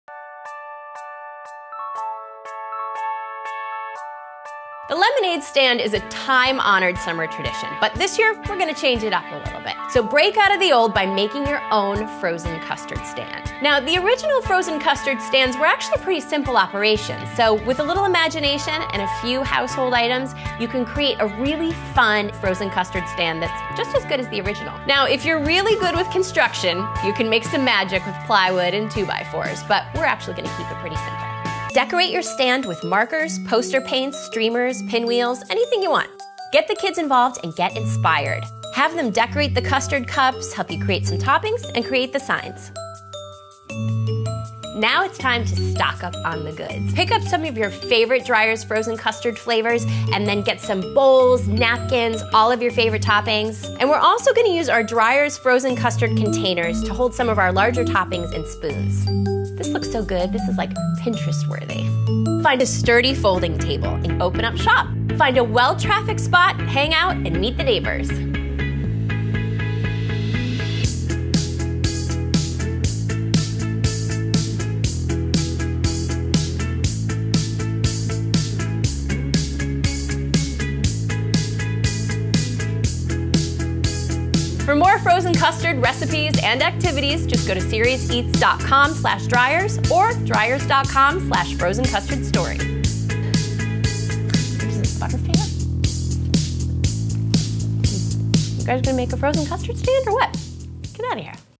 Launch a Frozen Custard Stand with Dreyer's and Serious Eats
Get creative this summer and upgrade your classic lemonade stand to a frozen custard stand.